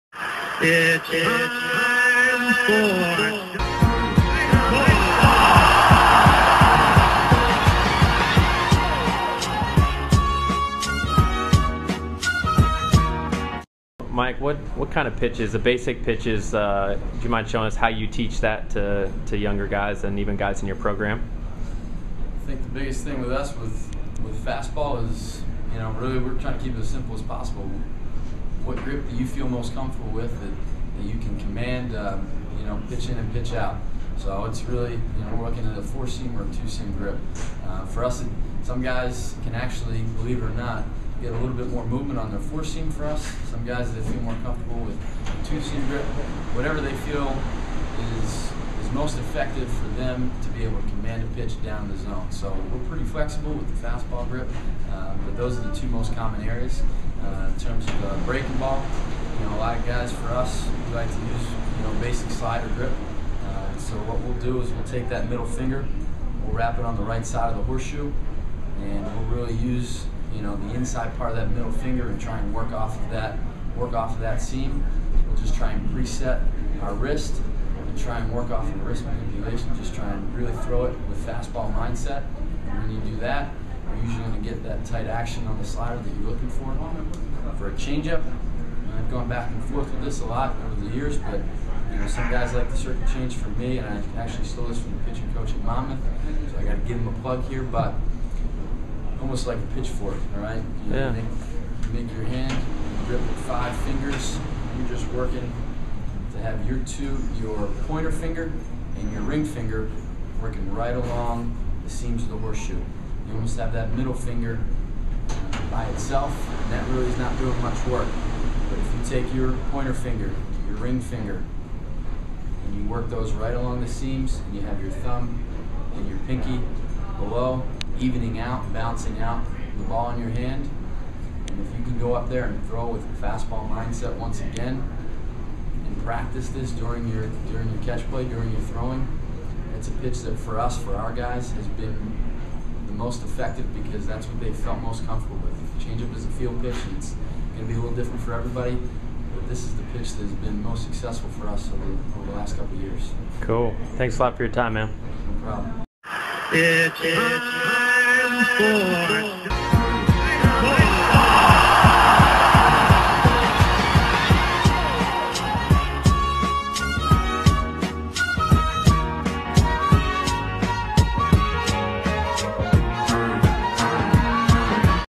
Pitching Grip Breakdown with Stony Brook Pitching Coach Mike Marron
Many thanks to Coach Marron for sharing his time and insights. I know the pitchers and coaches out there will enjoy this breakdown of the pitches used on one of the best Division 1 pitching staffs in the country.